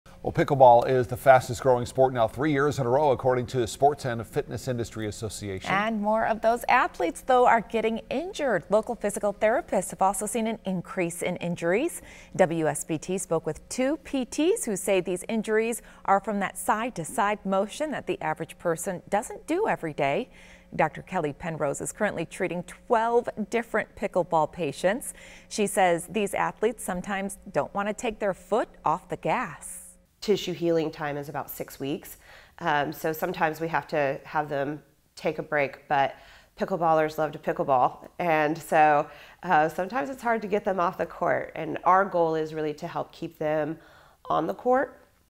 Pickleball injuries on the rise
Pickleball is the fastest growing sport for 3 years in row, according to Sports & Fitness Industry Association. As the sport has increased in popularity over the years, so have the injuries.